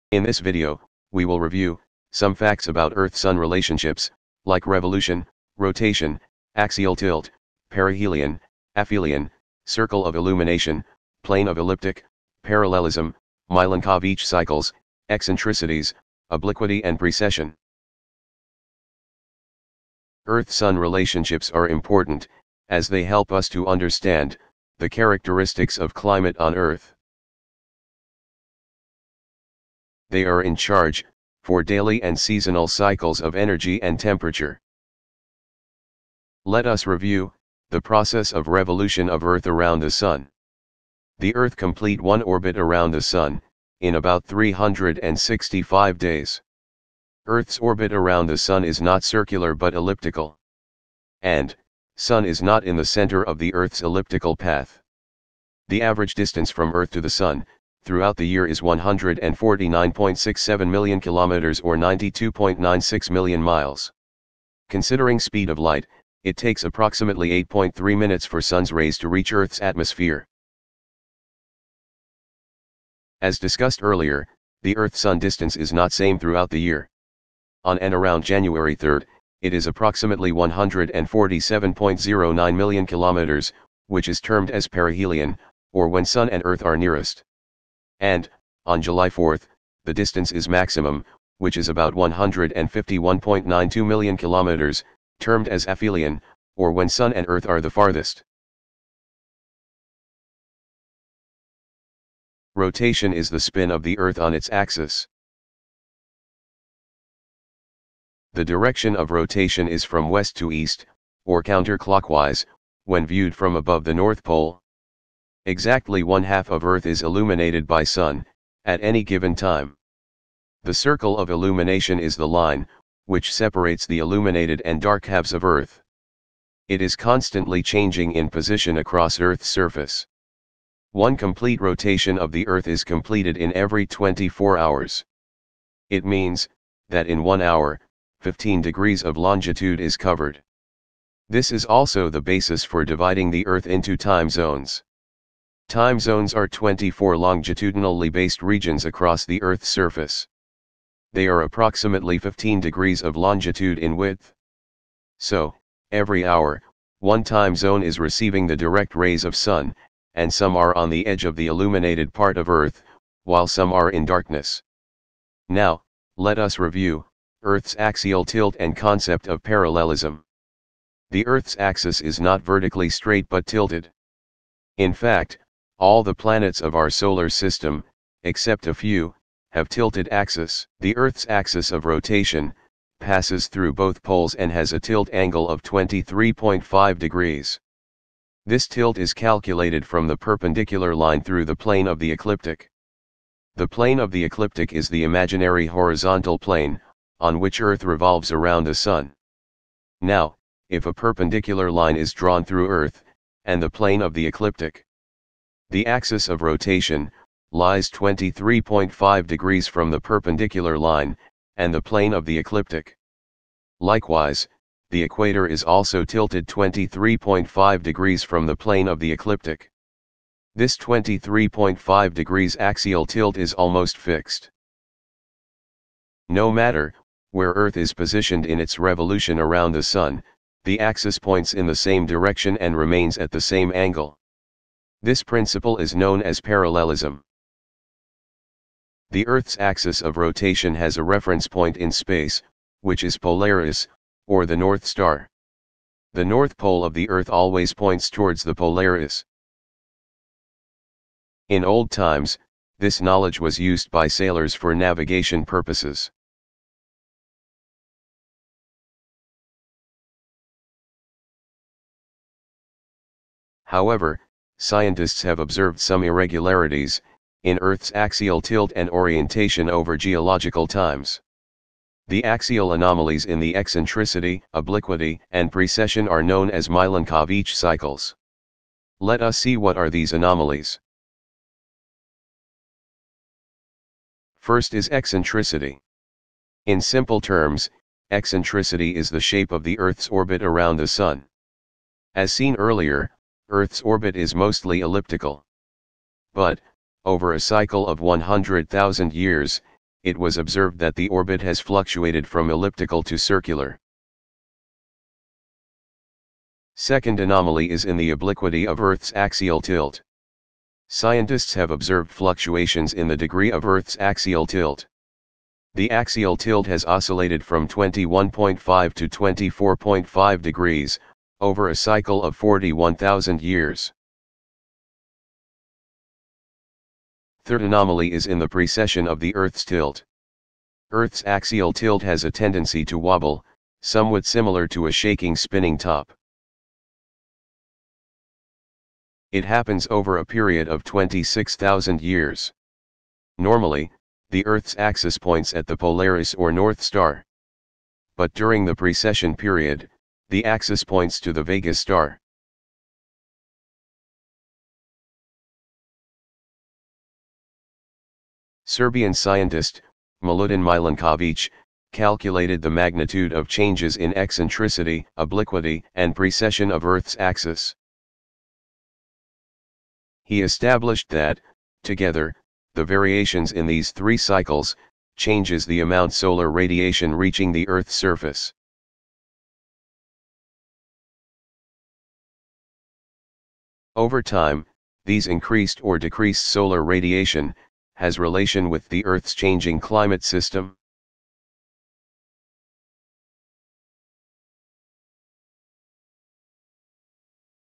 Some facts about Earth–Sun Relationships
In this video, we will review, some facts about Earth-sun relationships. Some facts , like revolution, rotation, axial tilt, perihelion, aphelion, circle of illumination, plane of elliptic, parallelism, Milankovitch Cycles, eccentricities, obliquity and precession, are explained with animation.

for interesting facts, posts about geography.